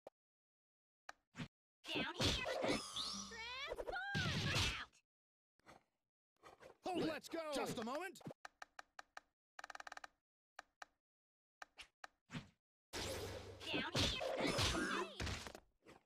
pea safe jump pressure idea
jhk here will probably just get PB'd out and is hard to setup but its still a threat and two pretty reasonablly fast overheads while being safe

jhk has 25 frames of block stun and non-perfect lowered airdash hits a frame before you land so 25-2 for landing lag slots perfectly into the safe jump on a lot of reversals